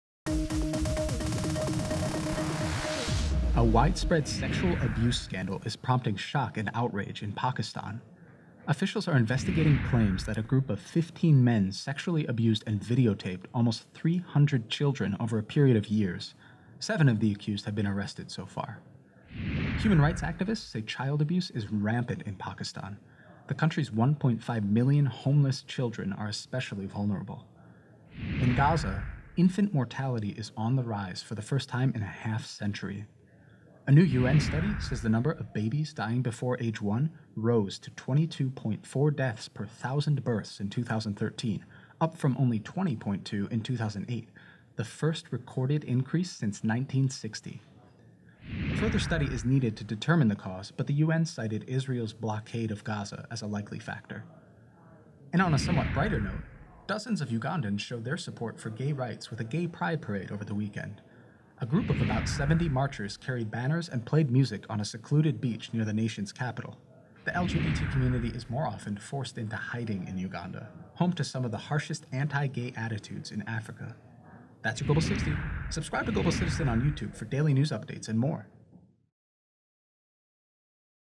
Global :60 - Sexual abuse in Pakistan, infant mortality in Gaza, and gay pride in Uganda - 8/10
The news you need to know, fast: A widespread sexual abuse scandal is prompting shock and outrage in Pakistan. In Gaza, infant mortality is on the rise for the first time in a half-century. And dozens of Ugandans showed their support for gay rights with a gay pride parade over the weekend.